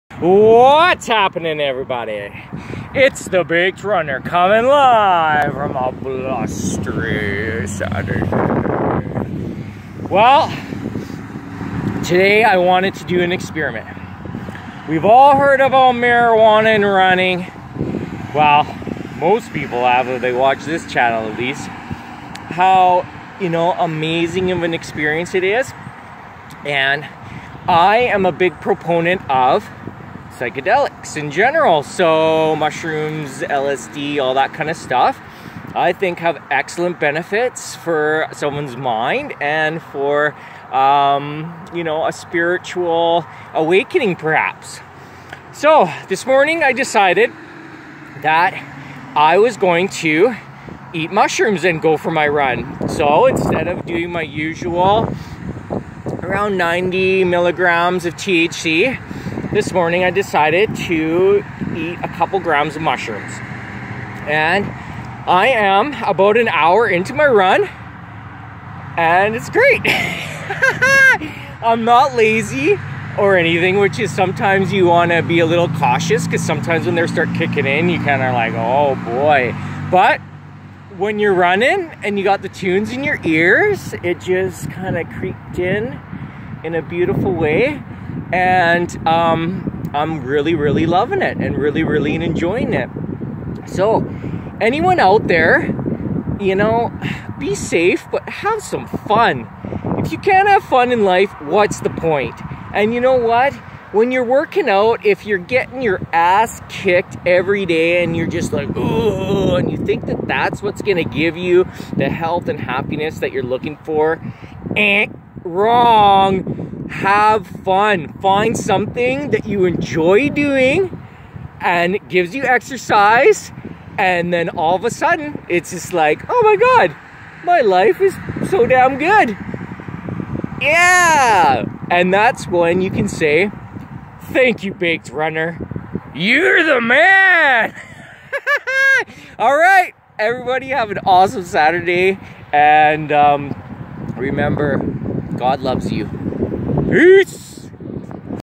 Magic Mushrooms and Running:)
If your bord of Marijuana and running, the next step could be magical:)